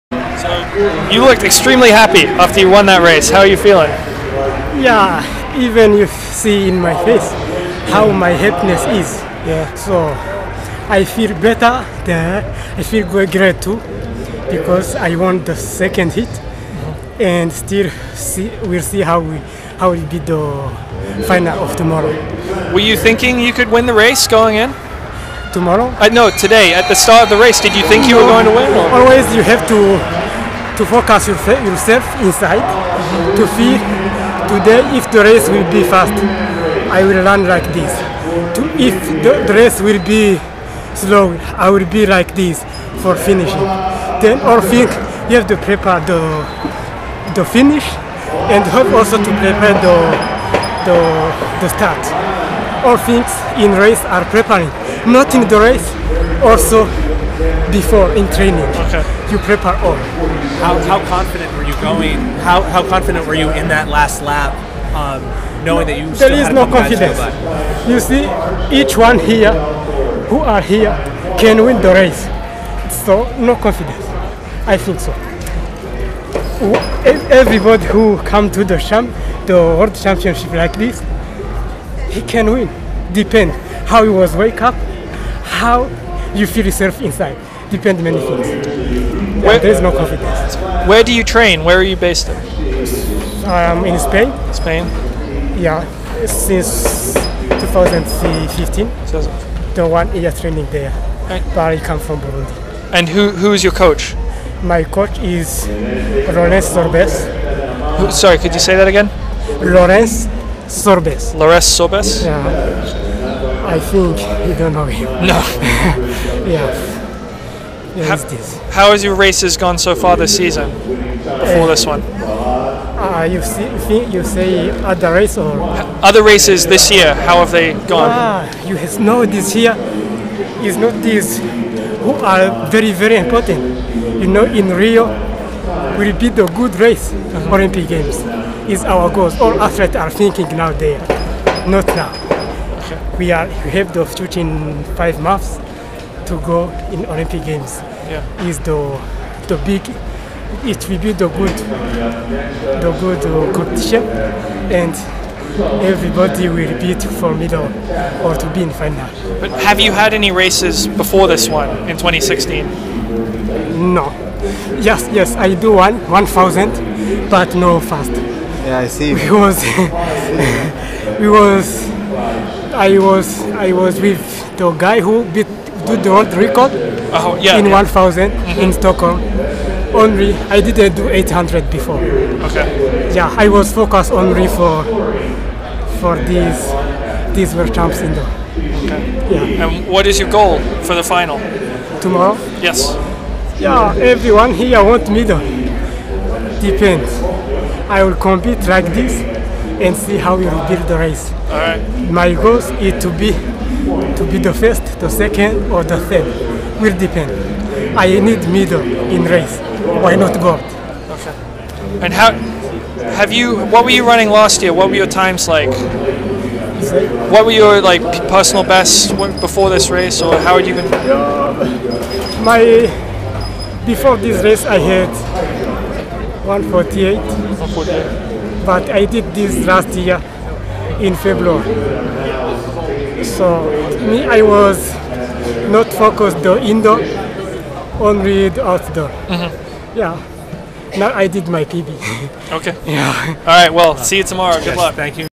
Burundi's Antoine Gakeme After Surprising in Heat 2 of 800 at 2016 World Indoors
LetsRun.com has more coverage of the 2016 World Indoor Championships